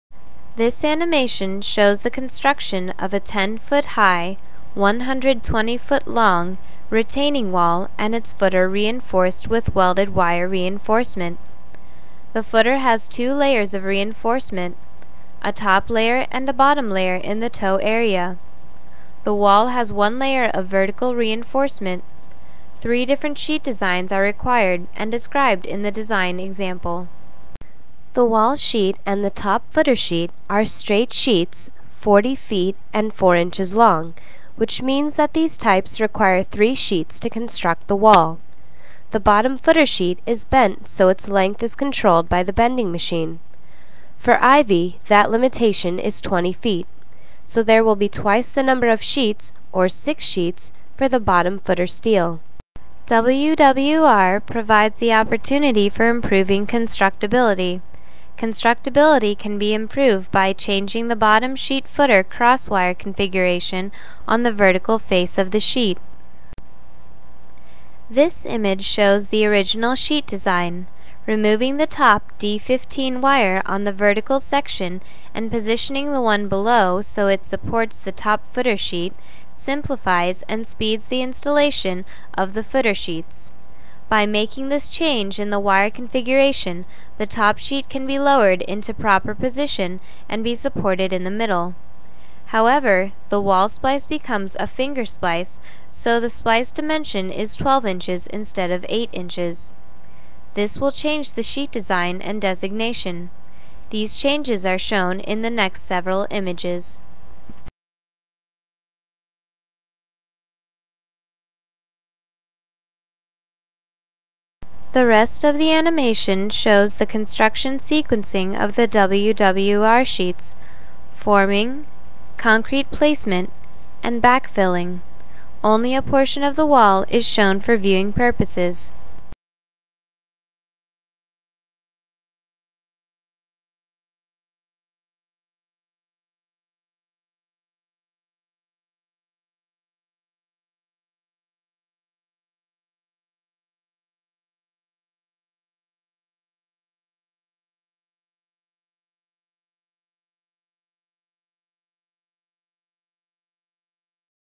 retainingWallAnimation.mov
This animation shows with the use of welded wire reinforcement (welded wire mesh) cast-in-place concrete retaining wall construction constructability improves and construction time is reduced.